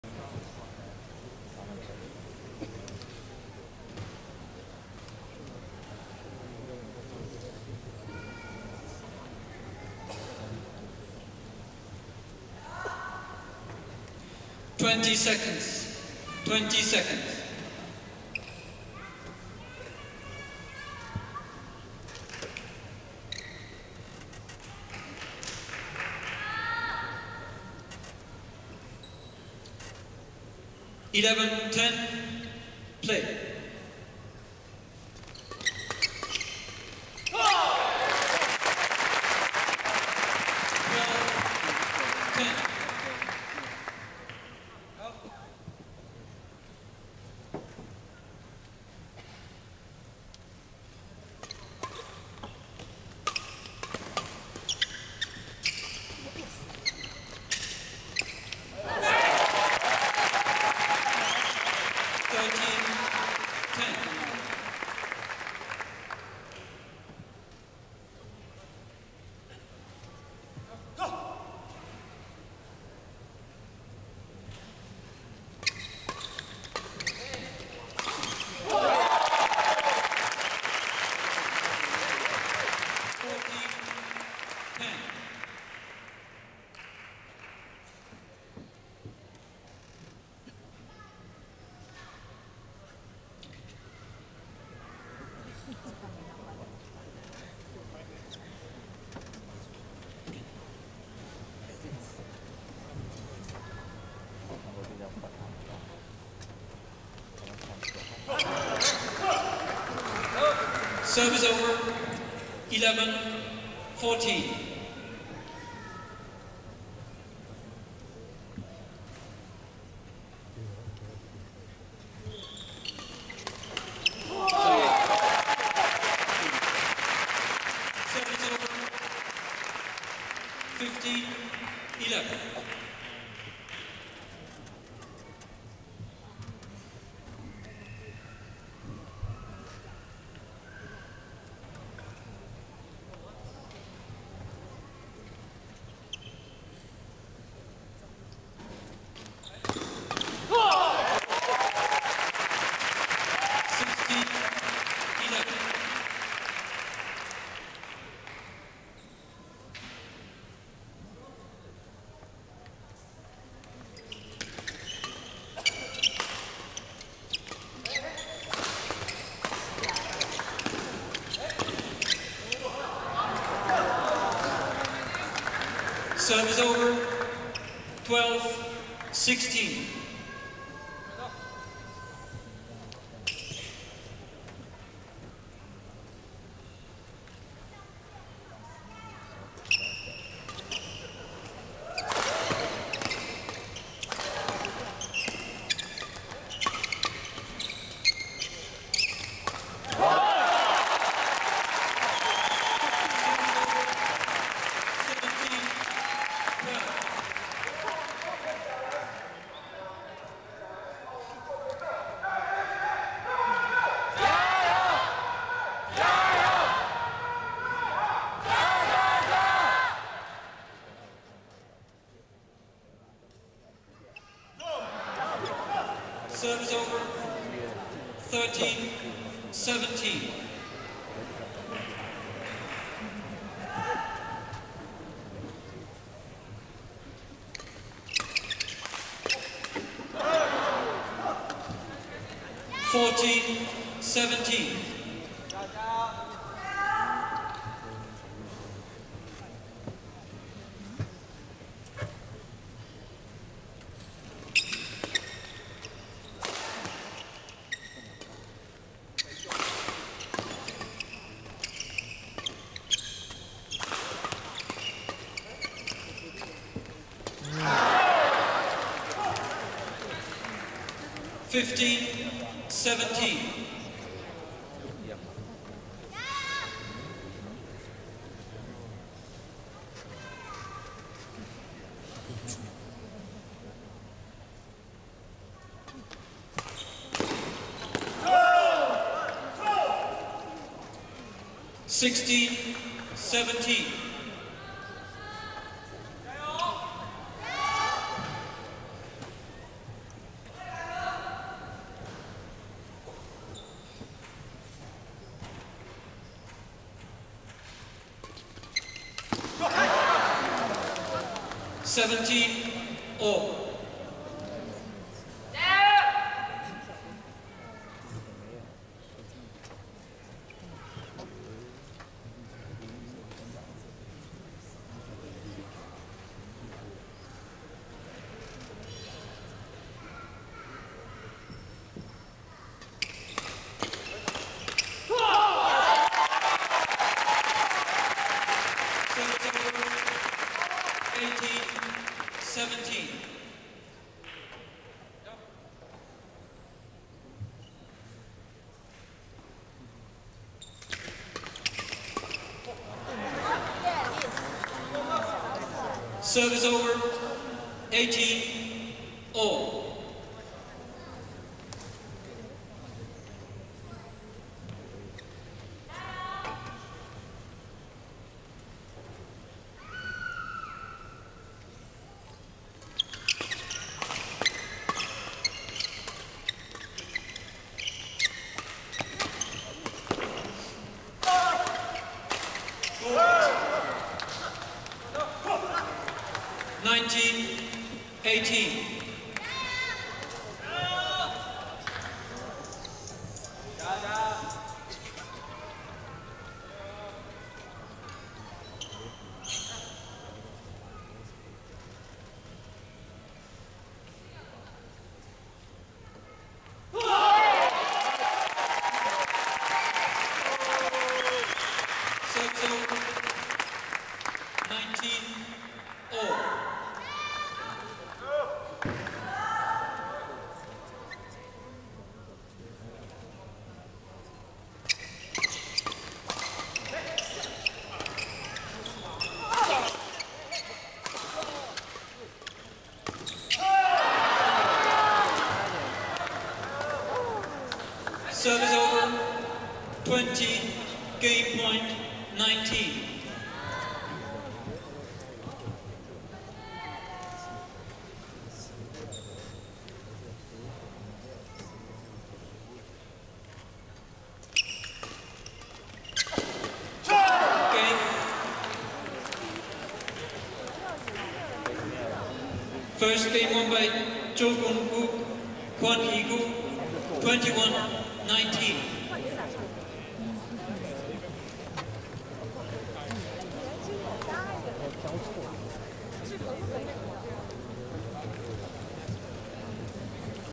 2010臺北羽球公開賽男雙四強 方介民/李勝木 vs. 曹建雨/權伊九G1-2
2010 Chinese Taipei Grand Prix Gold MD SemiFinals Chieh Min FANG & Sheng Mu LEE  vs. Gun Woo CHO & Yi Goo KWON